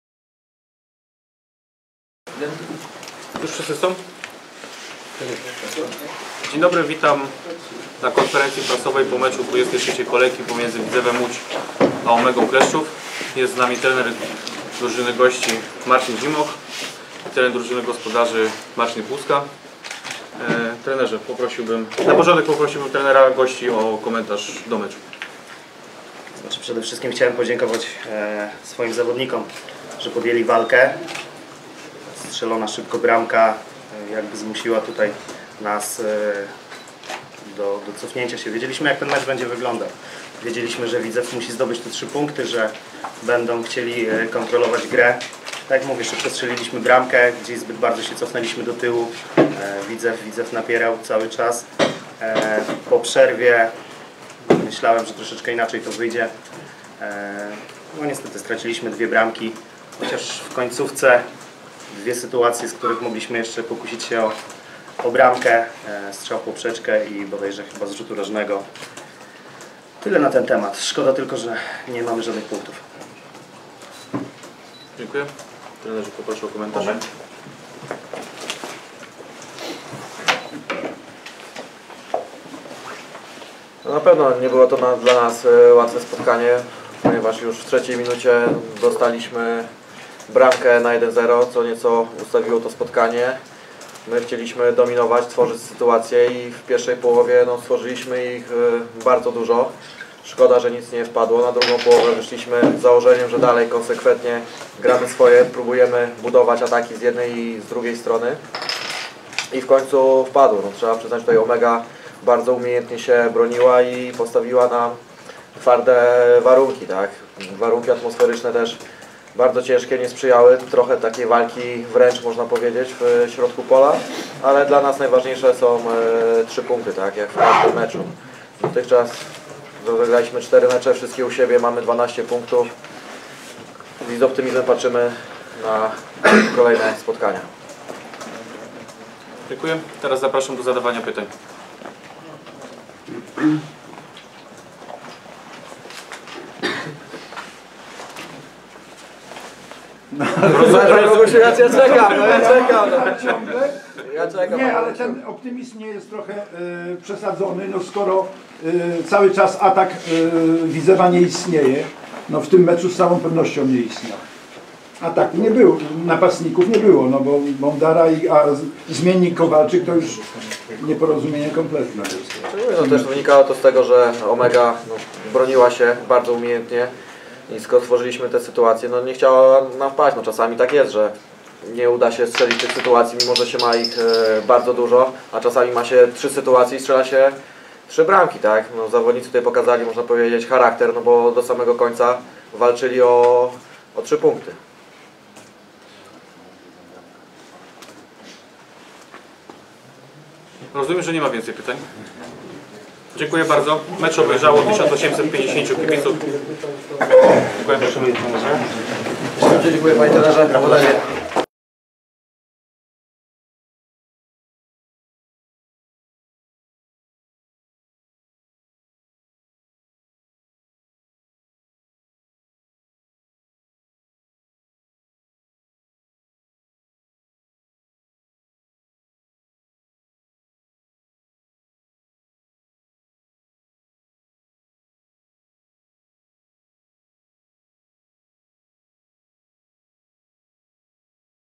WidzewTV: Konferencja po meczu RTS Widzew Łódź - Omega Kleszczów
Zapis konferencji pomeczowej.
Zdjęcia: Łukasz Kolecki